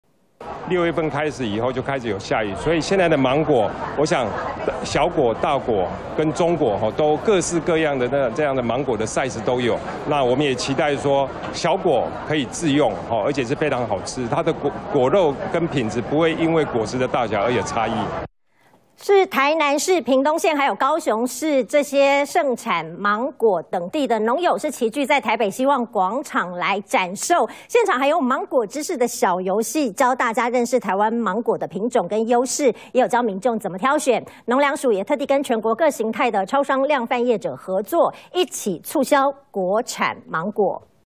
芒果盛產期又香又甜！鼓勵民眾選擇國產芒果｜三立新聞台
台灣芒果品種相當多元，7月也剛好是盛產期，農糧署積極向民眾宣導如何挑選芒果的技巧，來認識台灣芒果的品種和優勢。